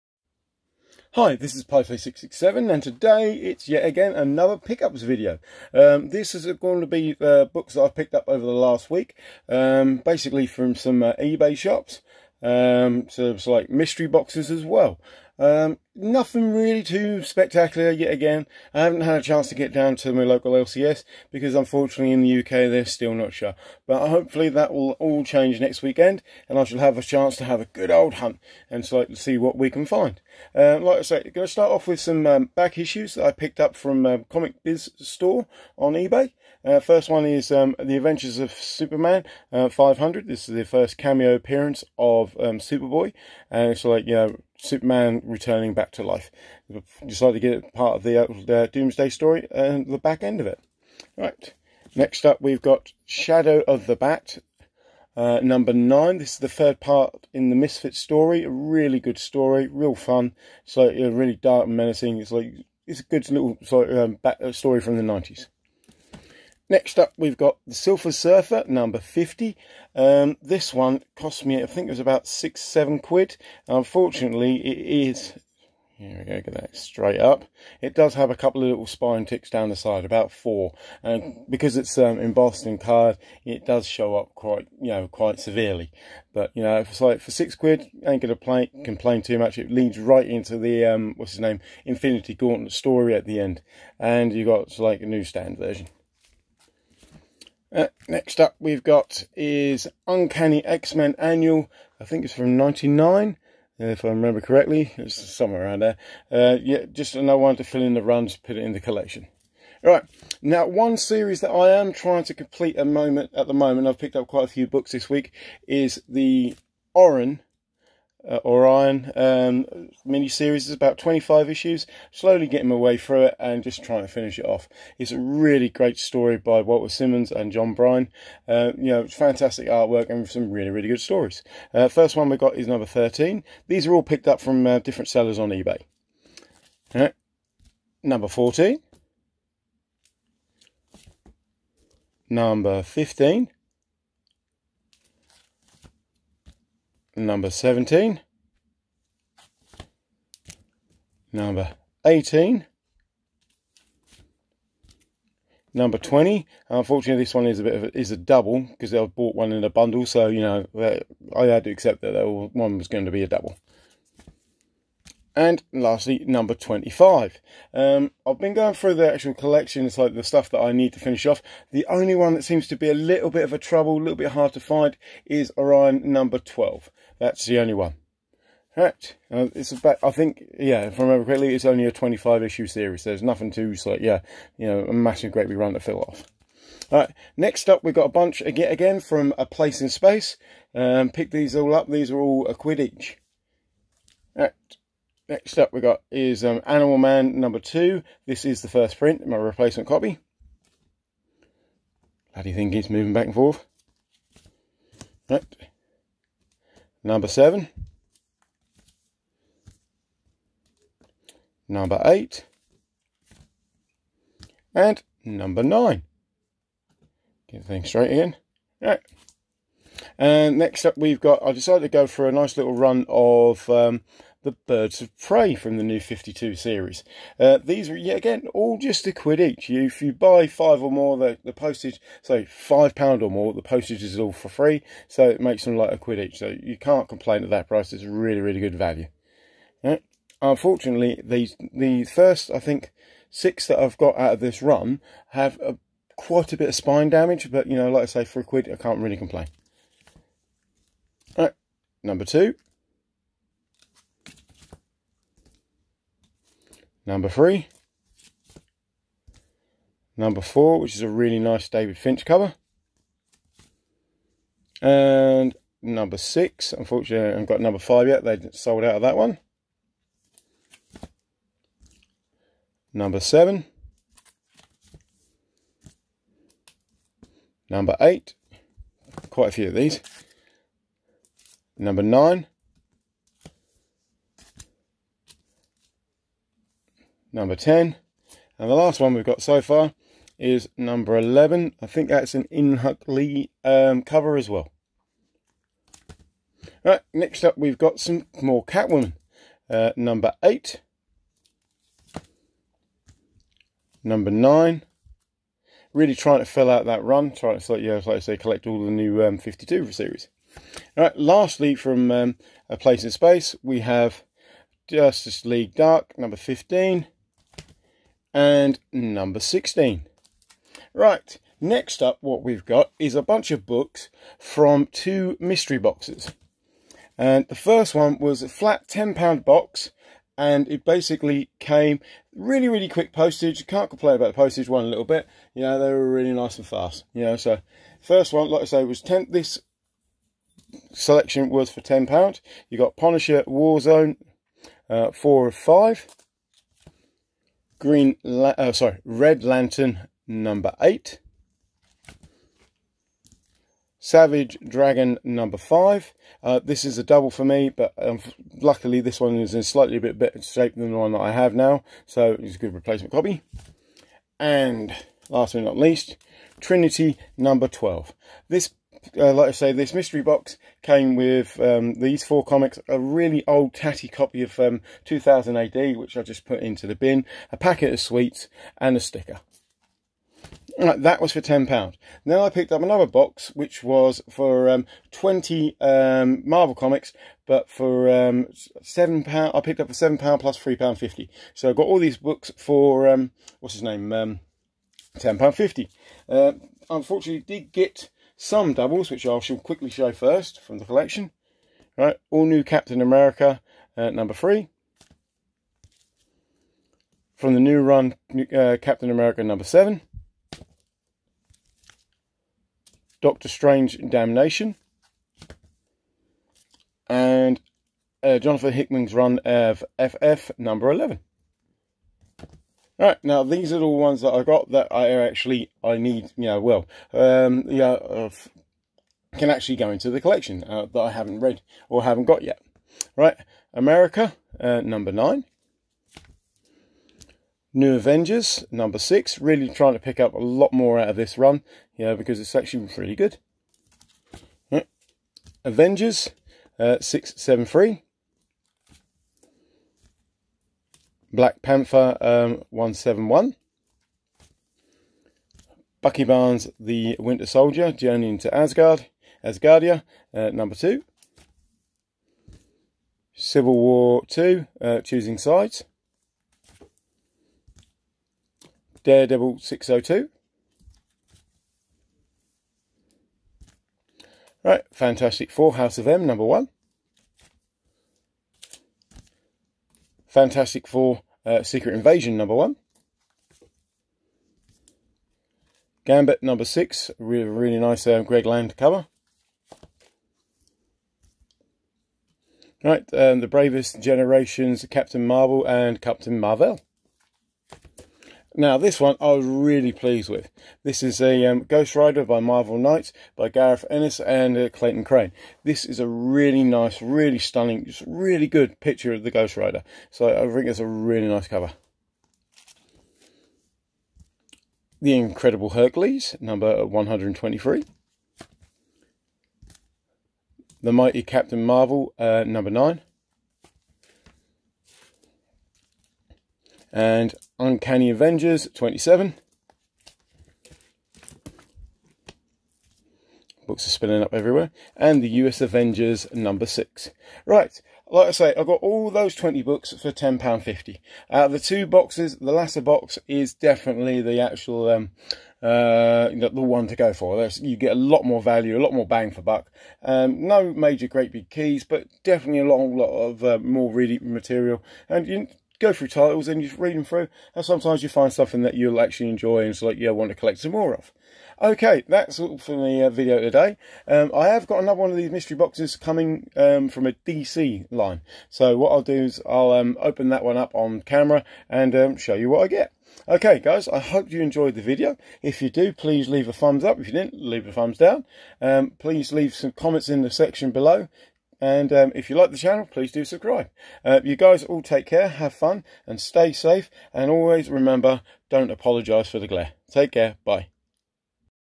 Comic Book Pick Ups #3. 14/06/20. Worst one on YouTube.
Here is a quick look at the comics I have picked up online over the last week.